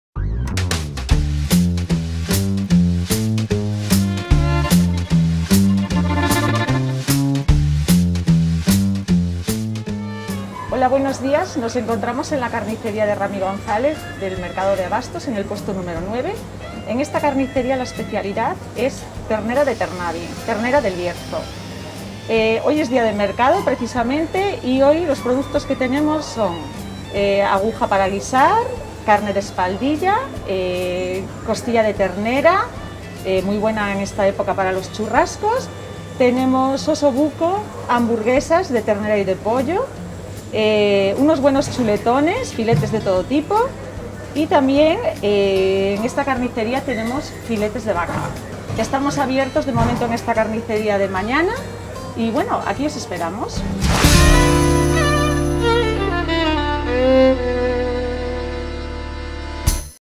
Carnicería Rami - Mercado de Ponferrada
En esta carnicería la especialidad es ternera de Ternabi, ternera del Bierzo.
Aquí os esperamos. 🏩